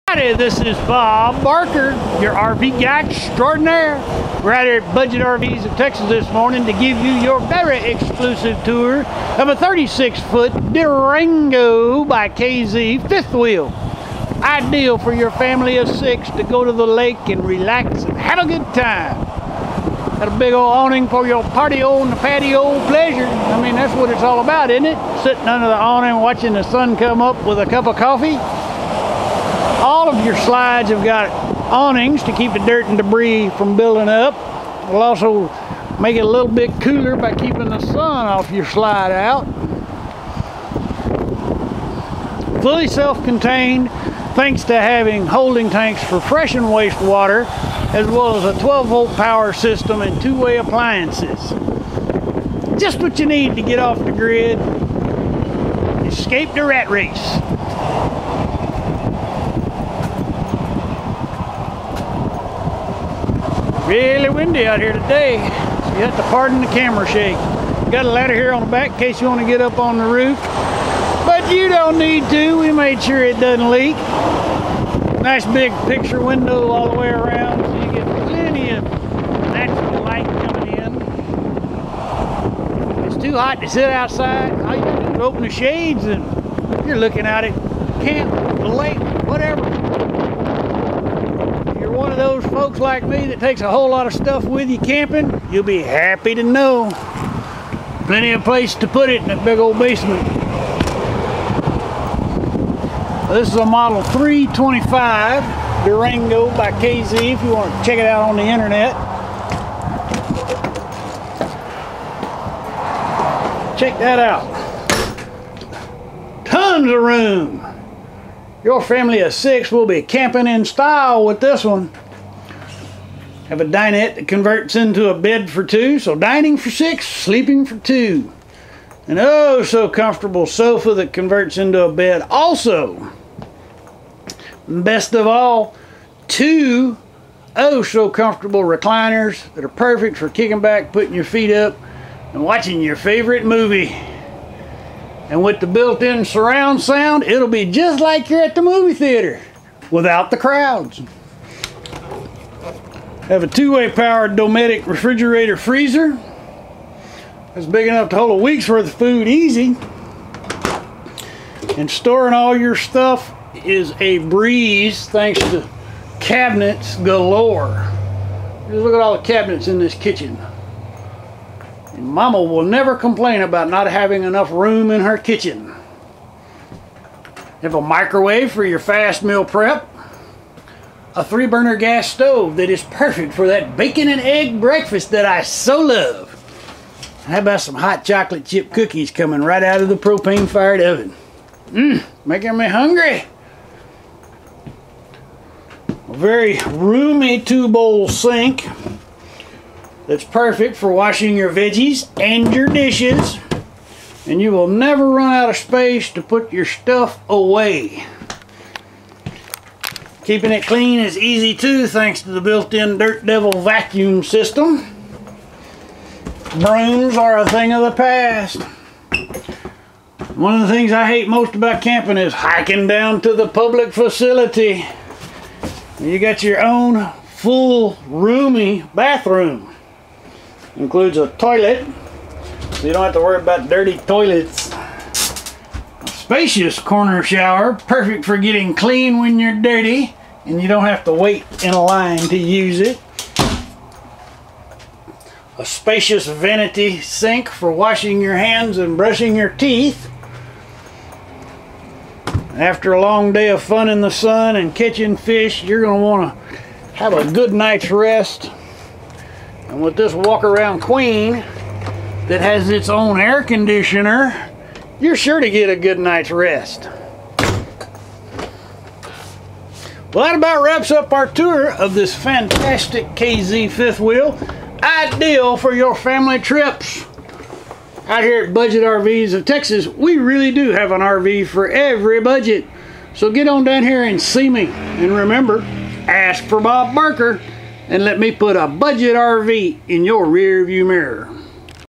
There is Still Time To Go Camping! Family Friendly 2008 Durango 325
The lakes are wide open! Now that summer has ended and everyone is back at work and school it is the perfect time to go to the lake! Just imagine all of the peace and quiet you will have! This easy to pull family friendly fth wheel is perfect for those long weekends at the lake cooking on the camp fire and eating S'mores! Thanks to the full bath you wont have to make those late night trips to the dirty public facilities! With a full kitchen great meals are fight at your fingertips! Holding tanks for fresh and waste water as well as a 12volt power system and two way appliances means you are fully self contained and can set up base camp where ever you want! With sleeping for six there is room for the kids to come along on your adventures! Priced for a quick sale at ONLY $22,995.00 this great family friendly fifth wheel wont last long! Call today to schedule your review , when you call remember to ask for Bob Barker and lets make a DEAL!